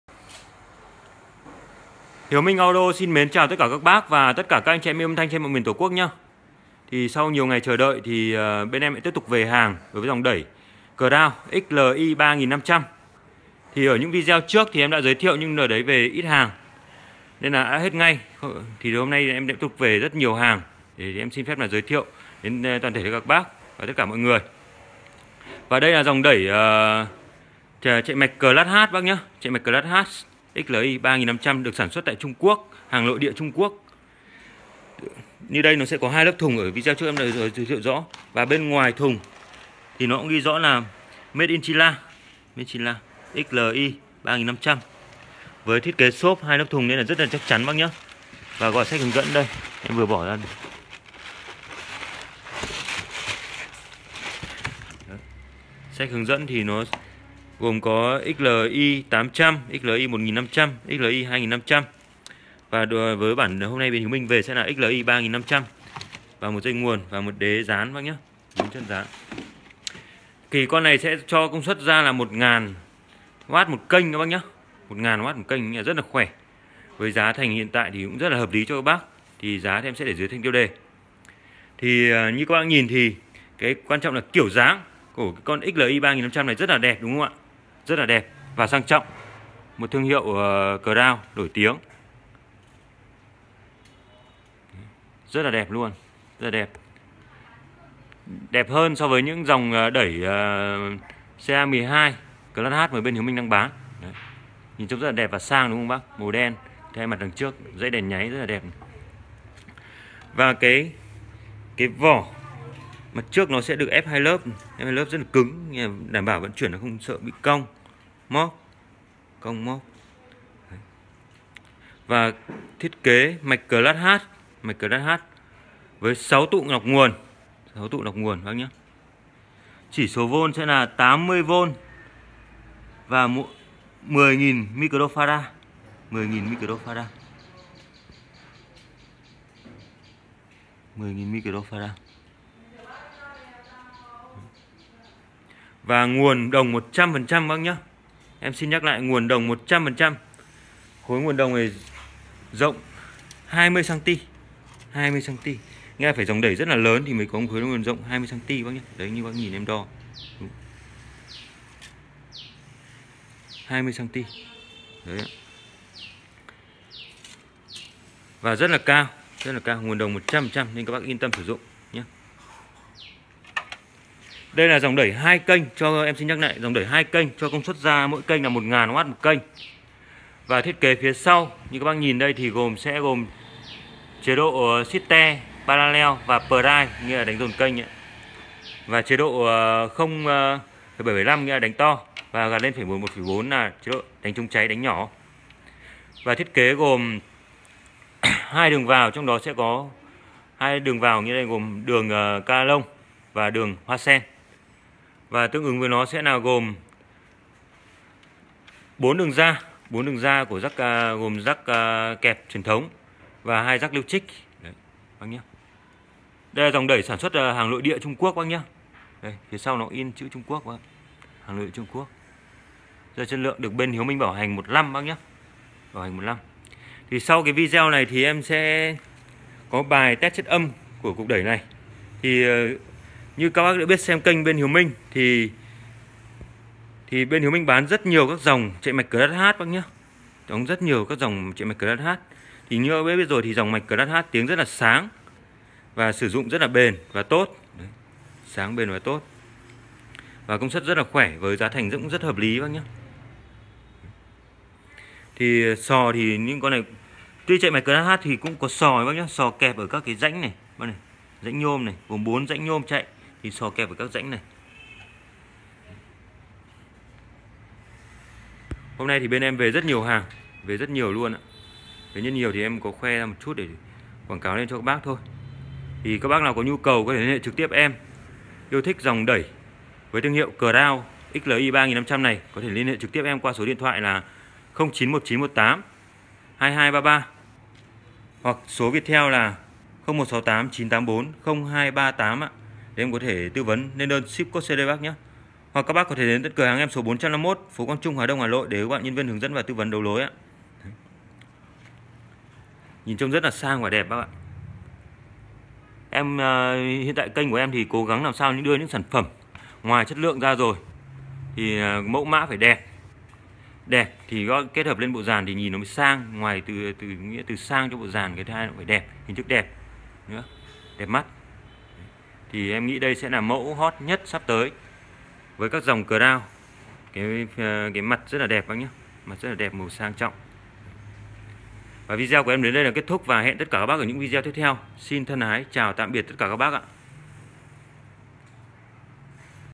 Cục đẩy Crown XLi 3500 (công suất 1000W/1kênh ) tiếng sáng.Giá 5tr000. LH:0919182233/0389840238
Cục đẩy công suất karaoke gia đình hai kênh công suất lớn Crown XLi 3500 chạy mạch CLass H tiếng sáng, trong trẻo.
Giá 5tr000
Địa chỉ:451 Quang Trung-Hà Đông-Hà Nội